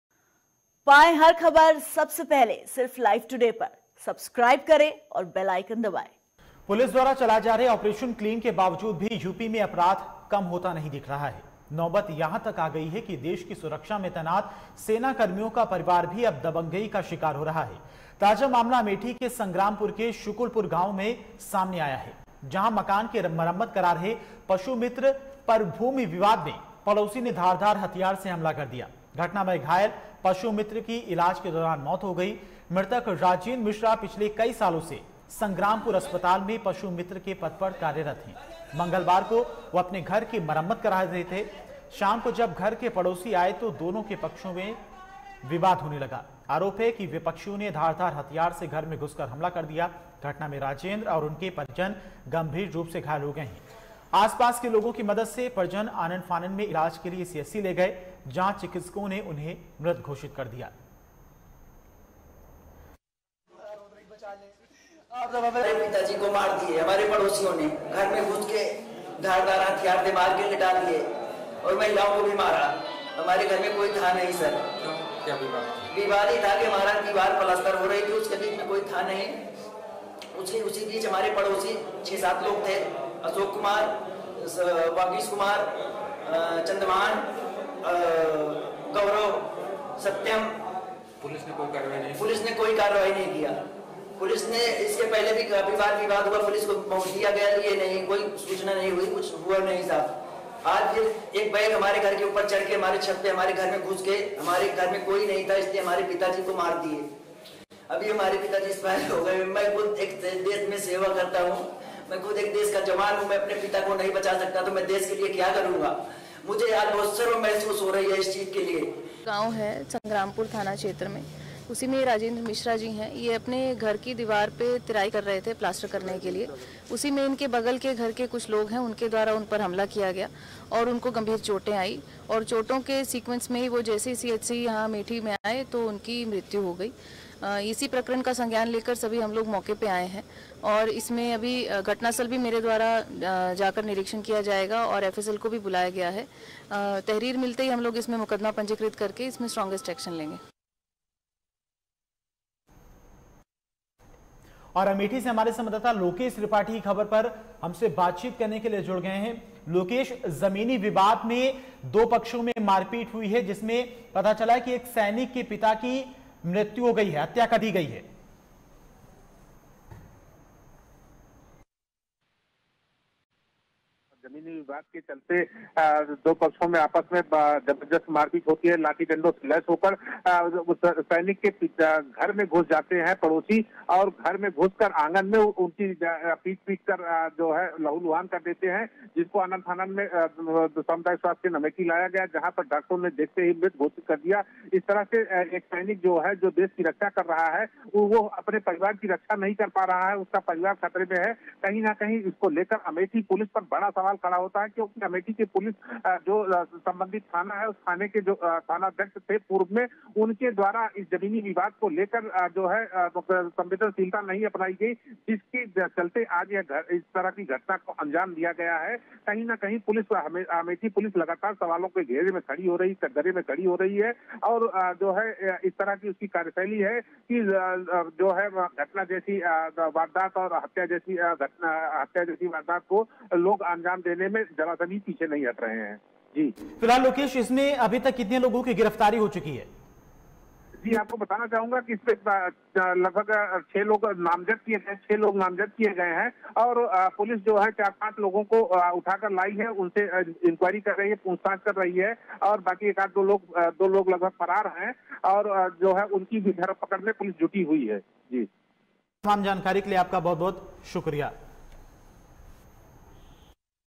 Amethi: सेना के जवान के परिवार पर बदमाशों का कहर, पशुमित्र पर बोला हमला
हमसे जुड़े रहने के लिए हमें फॉलो करें